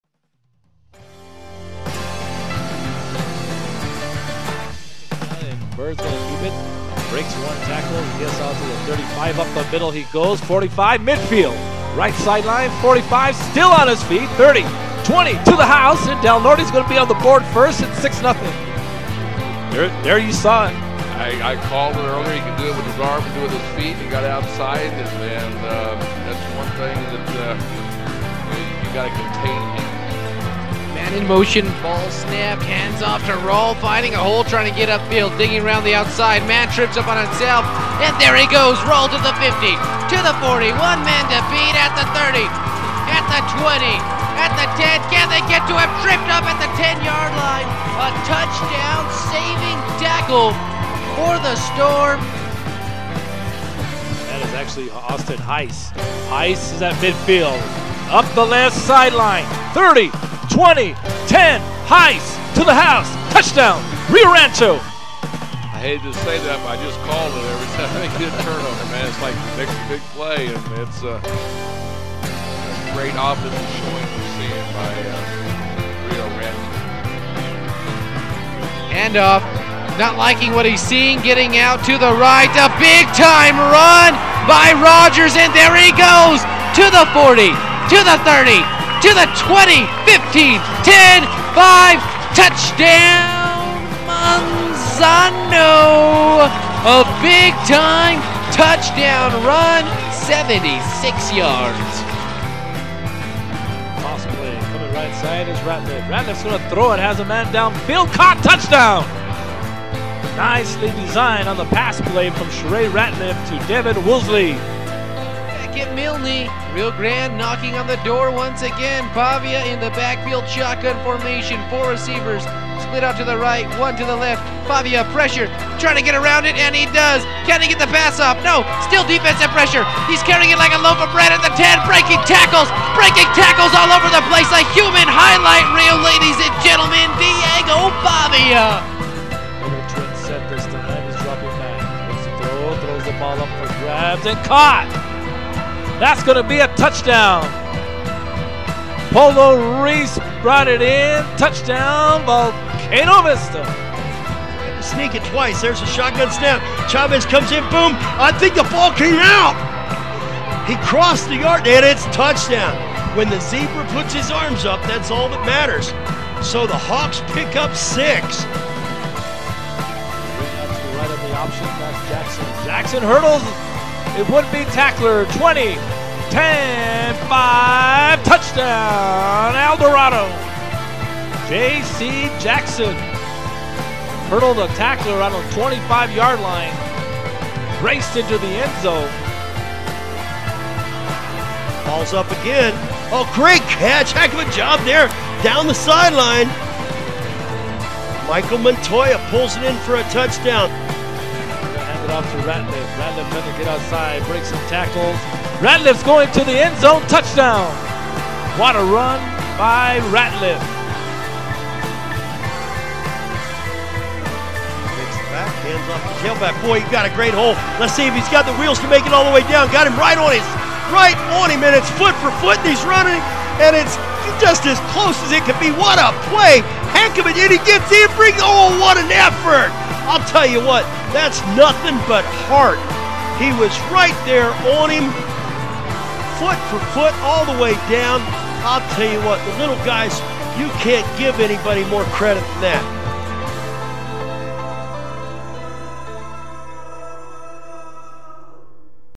YAFL Semifinal highlight
Highlights of Young America Football League semifinal action at WIlson and Milne Stadiums in Albuquerque, New Mexico, USA.